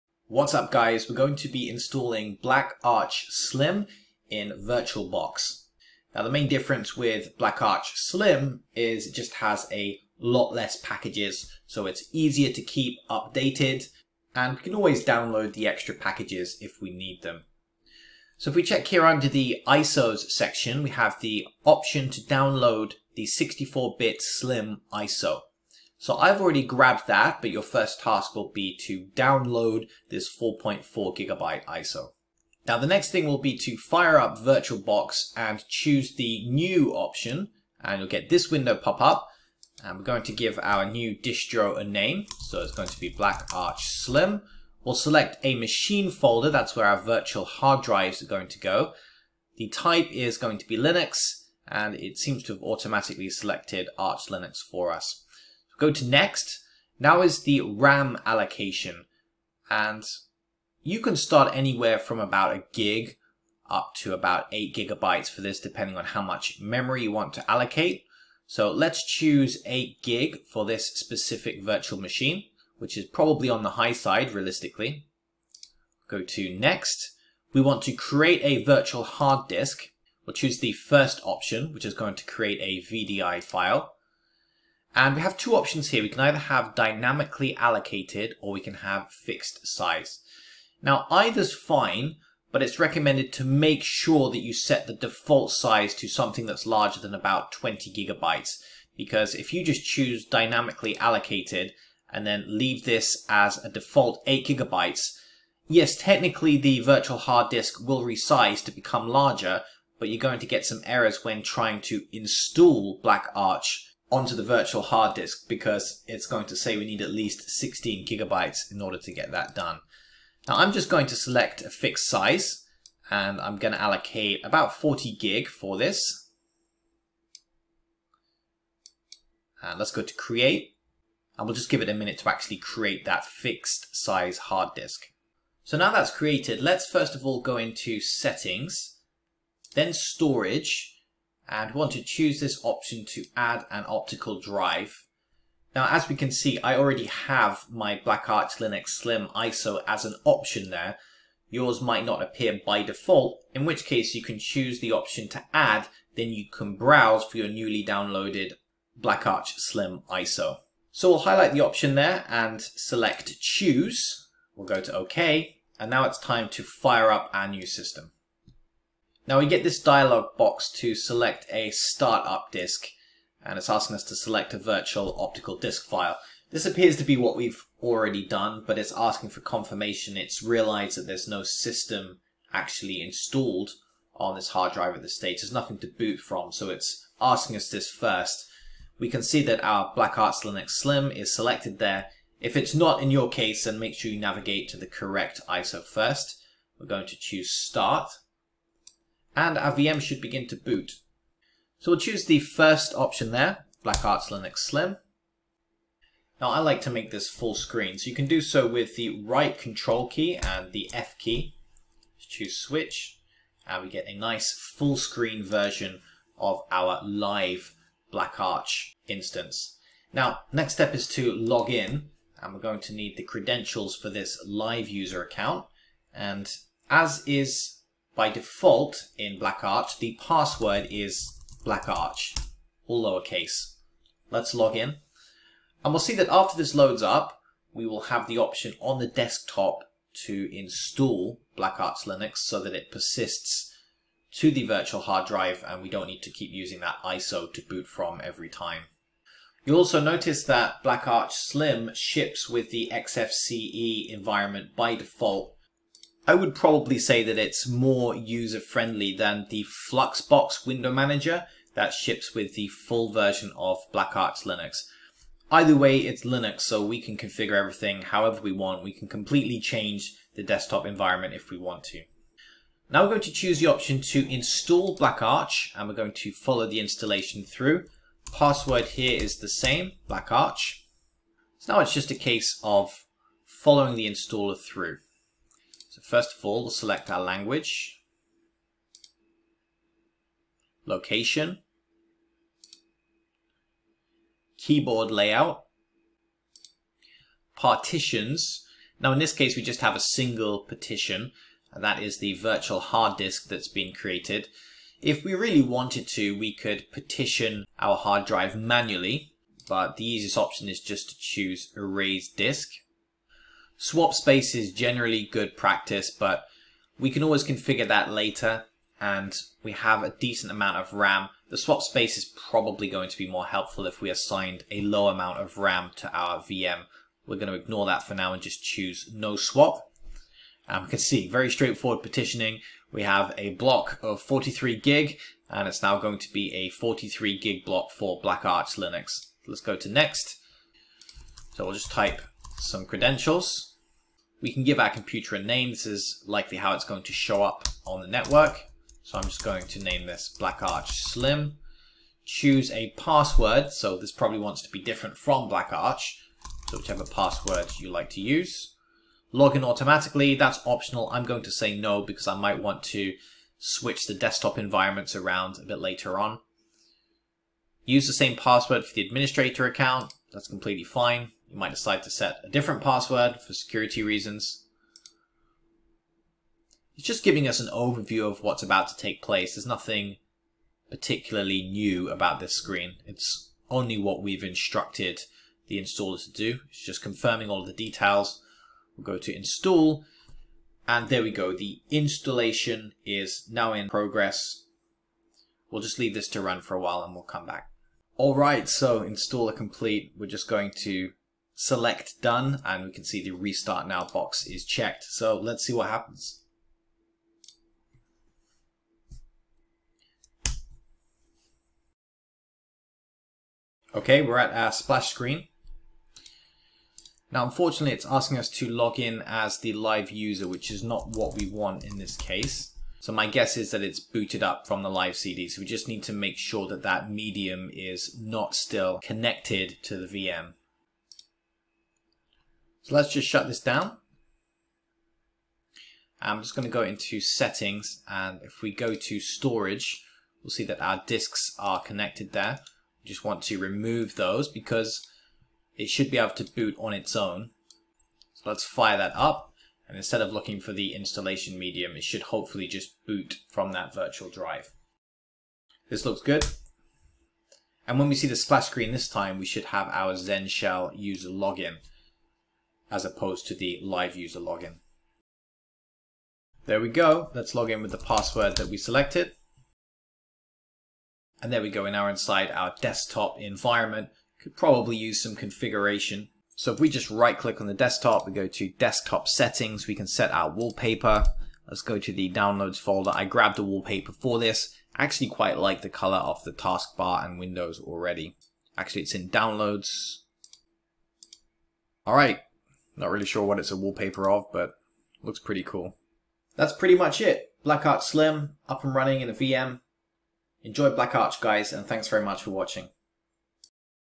BlackArch Slim Full Installation With Virtualbox
In this tutorial, we install Black Arch Slim edition on Virtualbox using an ISO image file.

00:00 Intro, differences and download
00:32 Create new virtual machine in Virtualbox
02:49 Firing up the new machine
04:32 Running the installer
07:15 Removing the ISO and booting BlackArch Slim